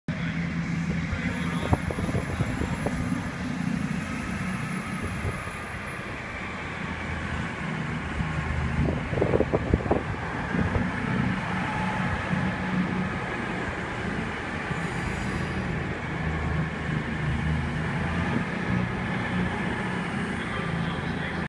221121
221121 departs Stafford for Bournemouth